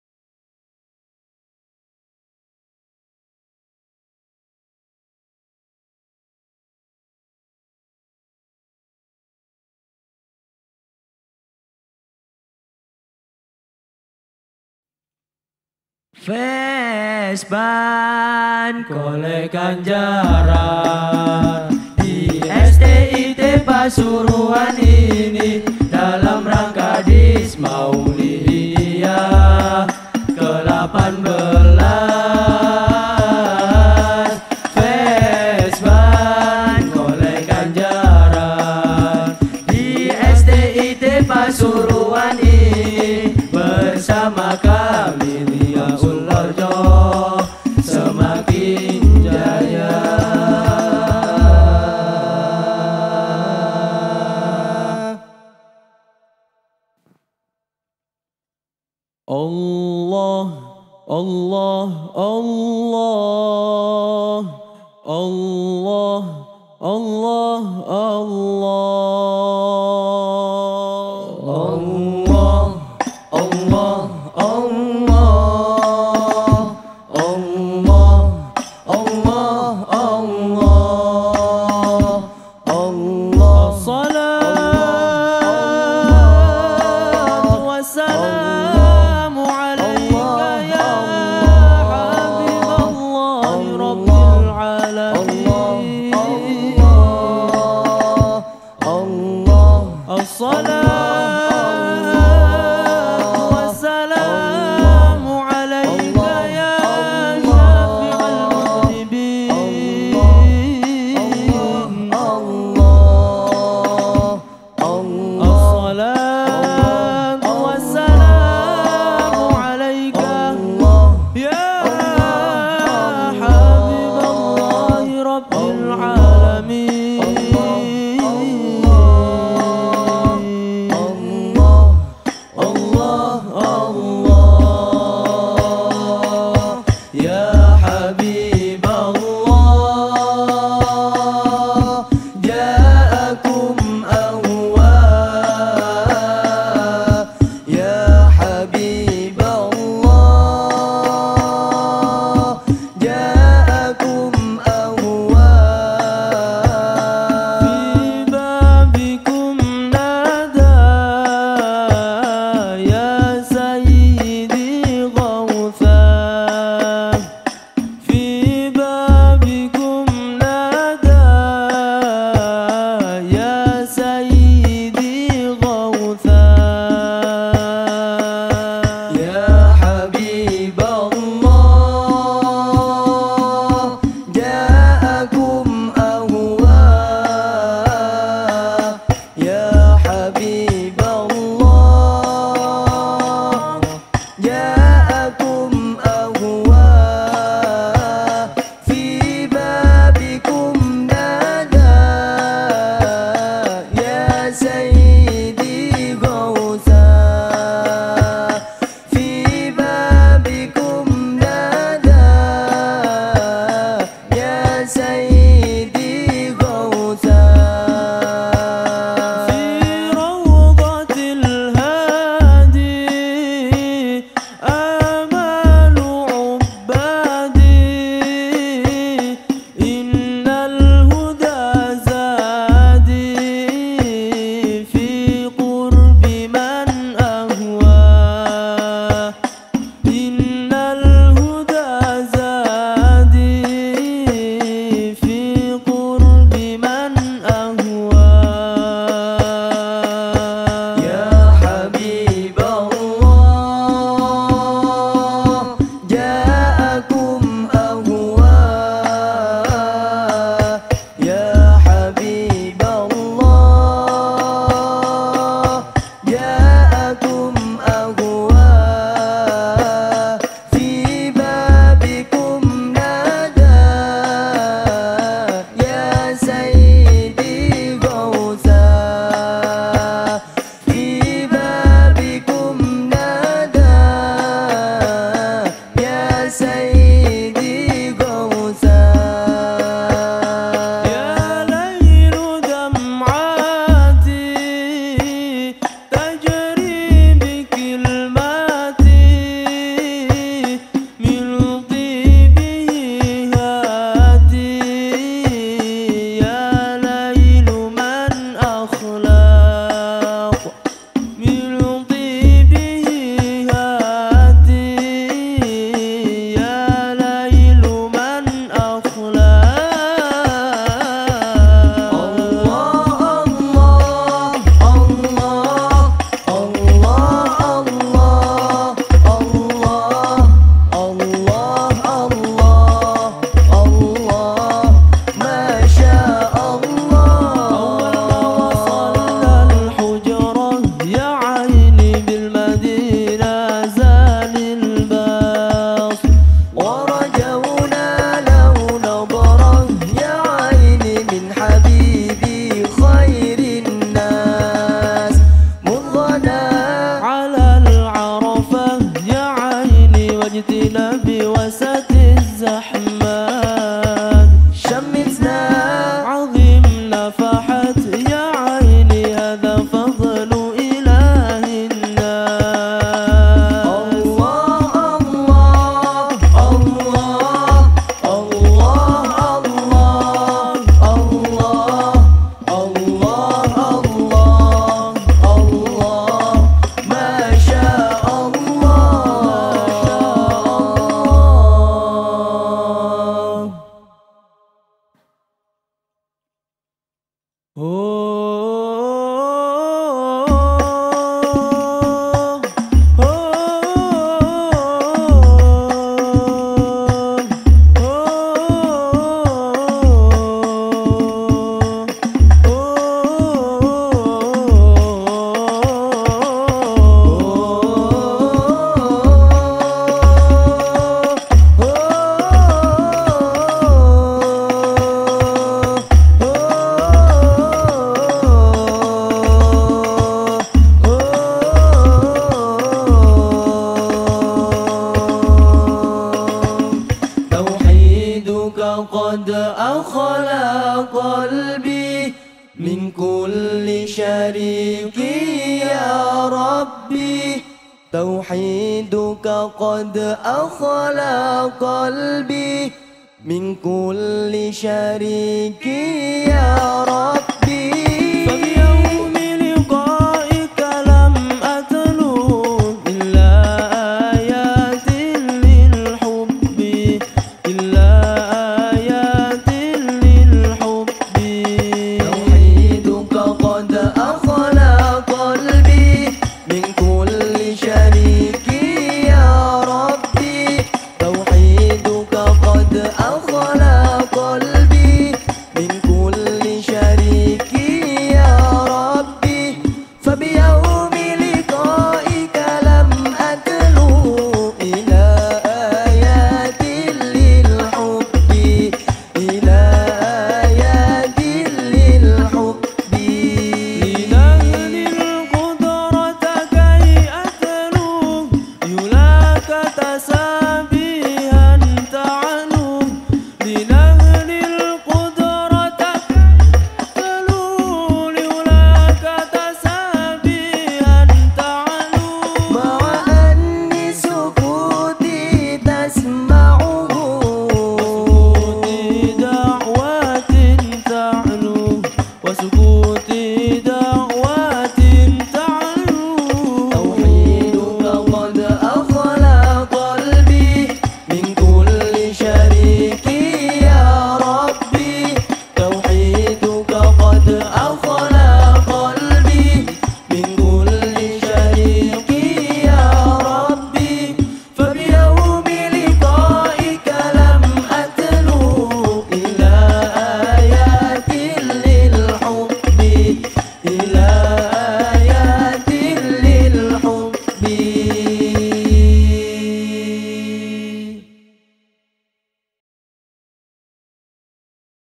MA Ma'arif Sukorejo | Fesban STIT PGRI Pasuruan 2023
Dokumentasi Festival Albanjari Se-Jawa Timur Ke-13
Tingkat SMA/MA/SMK Sederajat
Dalam Rangka Dies Maulidiyah STIT PGRI Pasuruan Ke-18

Gedung Serbaguna STIT PGRI Pasuruan